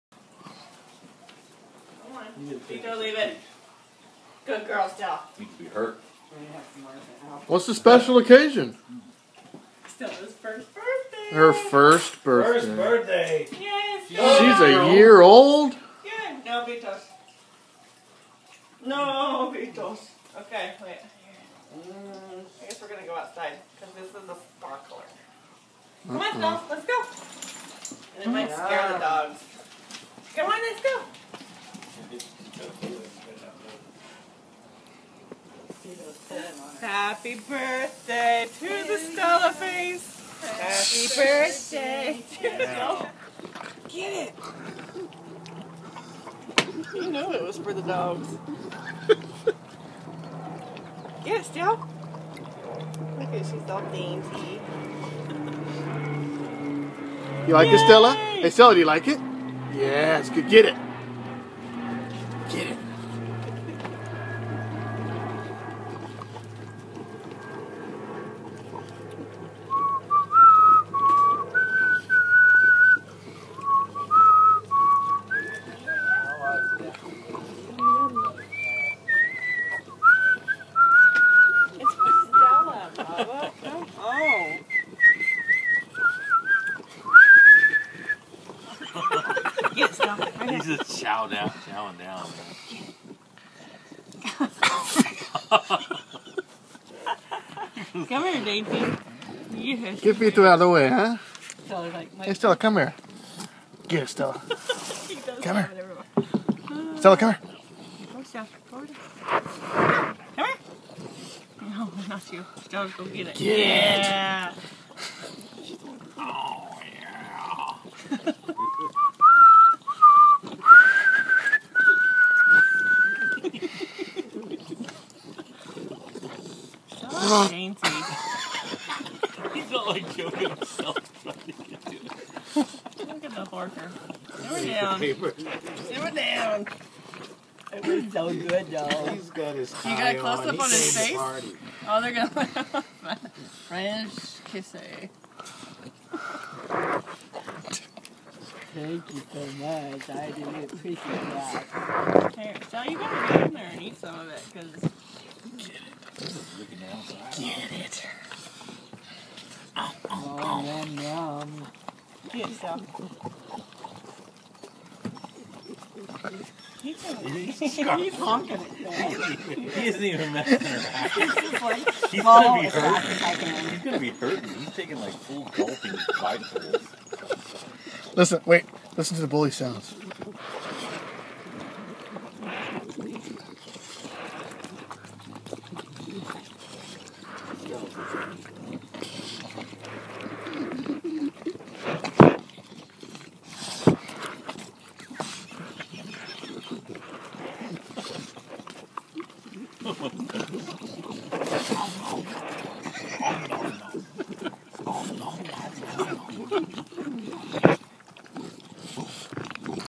Stella Bday
1 years old!!!  Stella eats cake and vito eats paper!